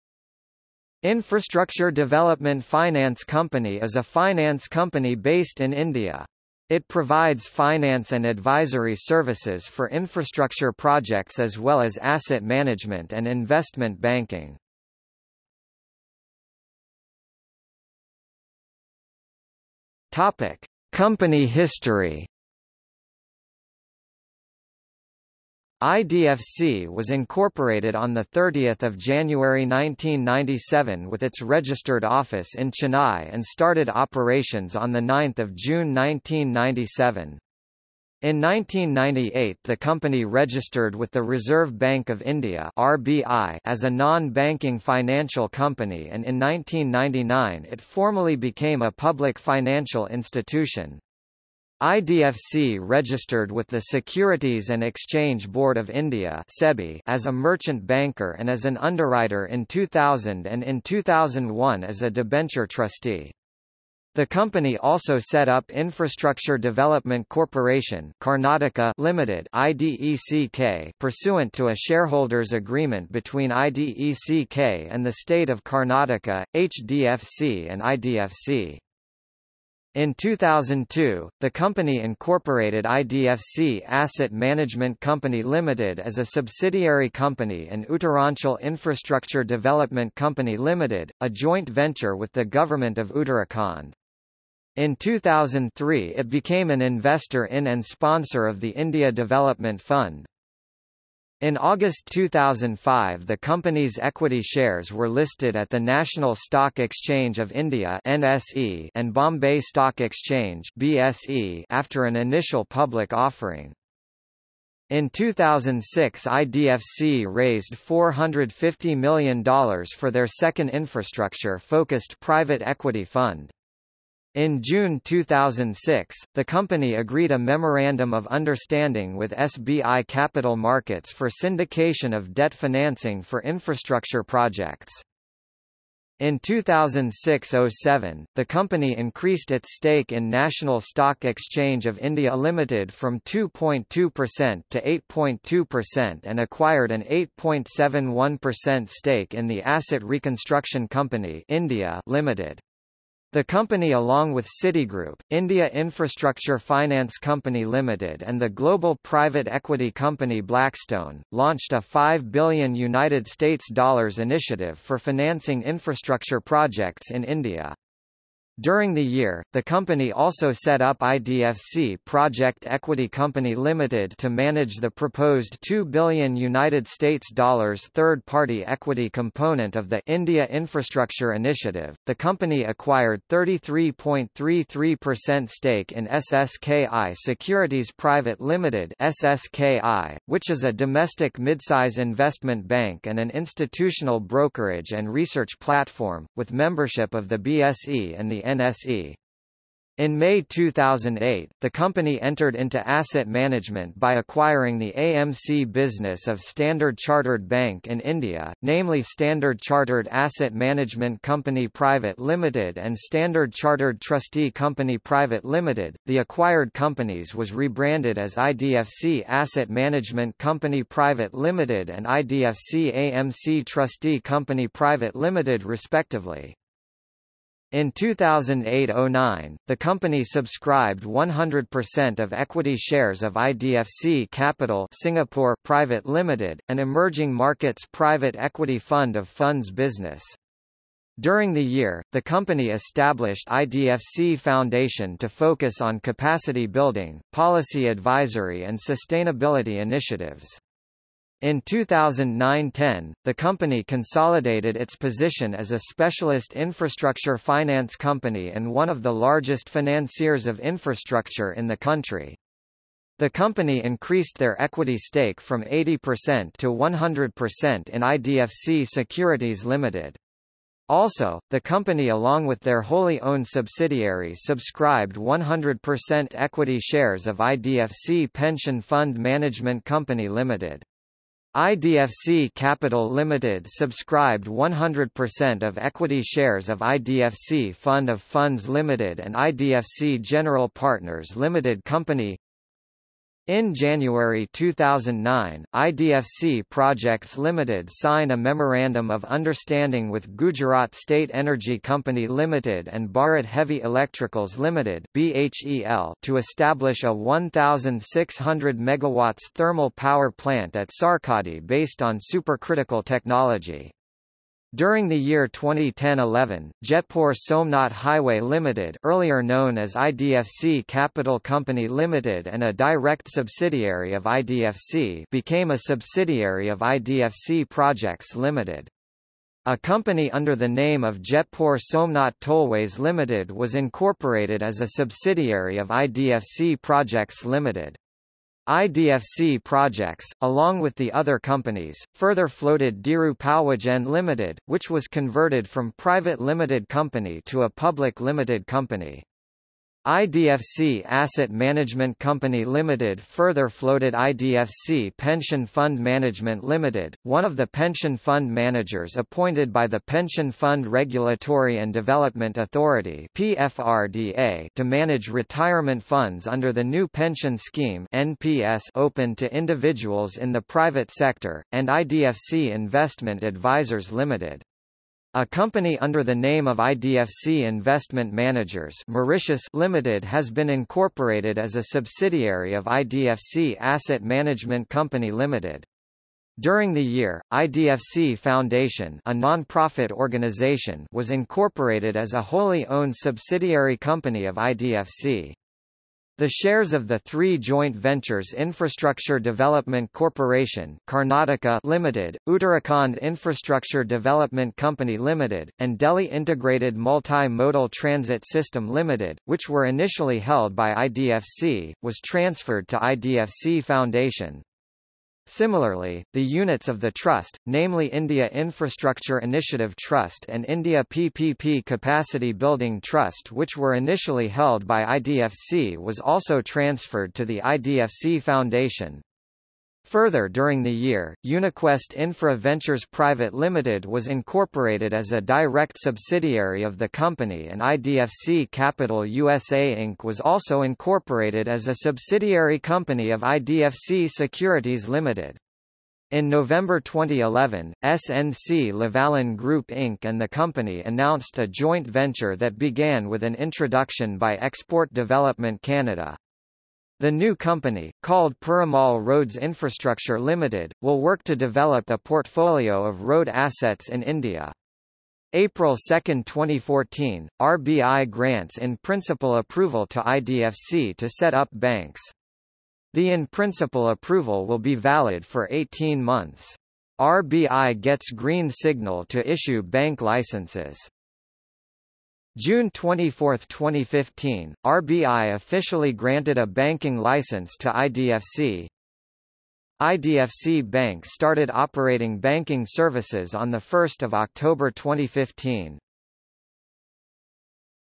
Infrastructure Development Finance Company | Wikipedia audio article
00:00:16 Company history

- Socrates

SUMMARY
Infrastructure Development Finance Company is a finance company based in India. It provides finance and advisory services for infrastructure projects as well as asset management and investment banking.